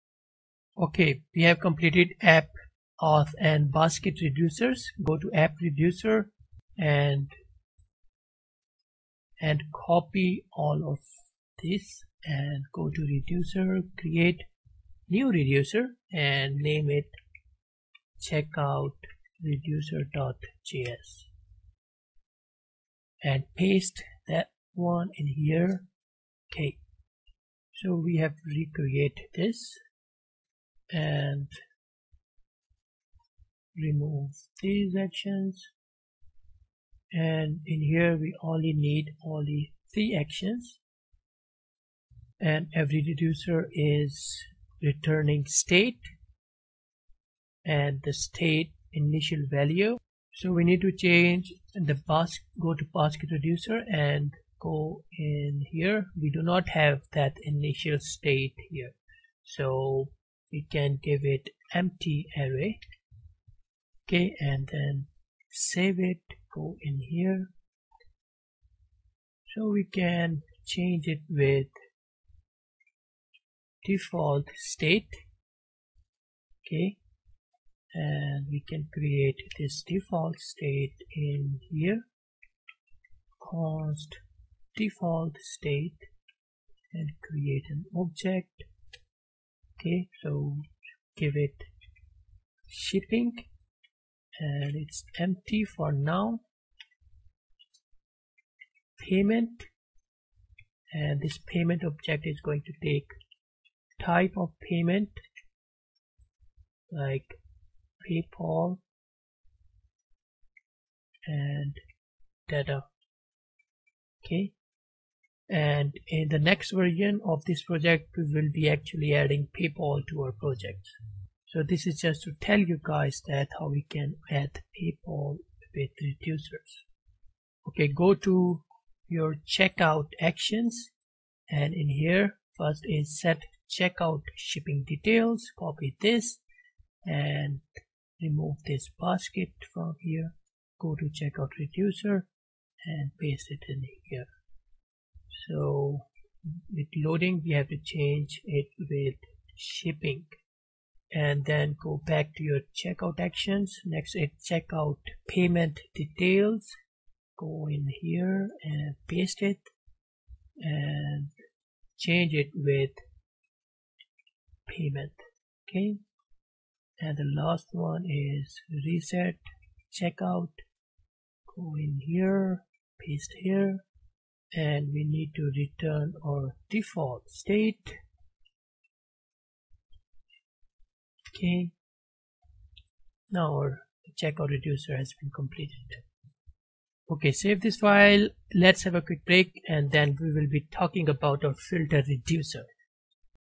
Tutorial#23|React.js/Redux E-Commerce - Writing CheckoutReducer
This is an React.js e-commerce project with Redux, babel, saga, node sass, firebase.
In this video we will be developing full e-commerce project with React and React tools mention above.
This project will add different Electronics products with shipping details.
Features
1. Admin Panel
2. User's Panel
3 Edit Account
4. See Orders
5. Different Filters
6. 2 Step verification and place orders
7. Profile and cover image
8. Firebase
9. Basket Items Handling
10. Cool Design with SASS
11. HTML
12. CSS
13. Bootstrap